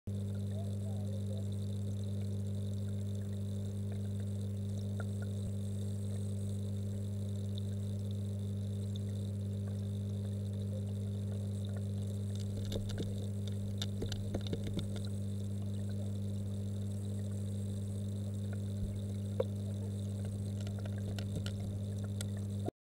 Dog Watching Wildlife in Rock Pool Tank
It's not just people who enjoy the rock pool tank at the Braunton Countryside Centre! We were testing out a Go Pro for our new interactive display when this happened!